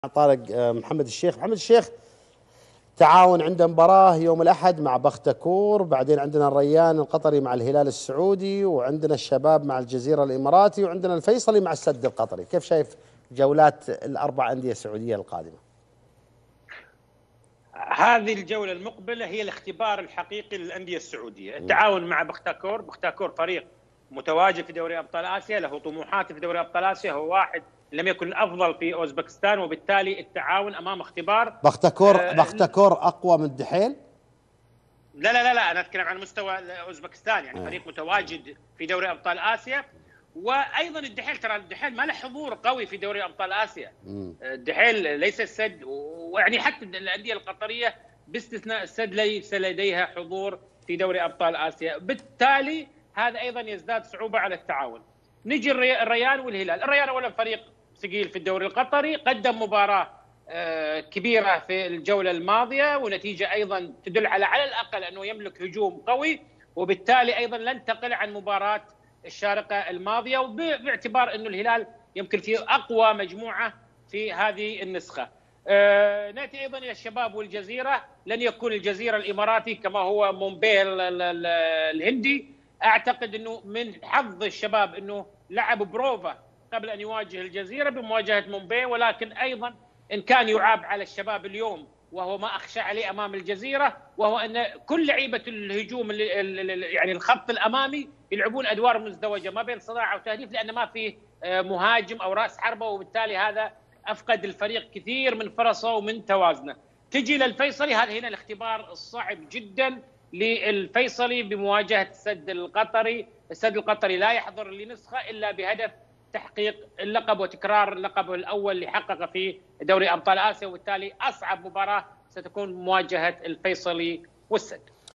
محمد الشيخ : الجولة القادمة هي الاختبار الحقيقي للأندية السعودية في آسيا ..
محمد الشيخ : الجولة القادمة هي الاختبار الحقيقي للأندية السعودية في آسيا .. الهلال في مواجهة الريان لا تقل صعوبة عن الشارقة .. والشباب كان واضح عليه حاجته للهداف .. والاختبار الأصعب للفيصلي في مواجهة السد .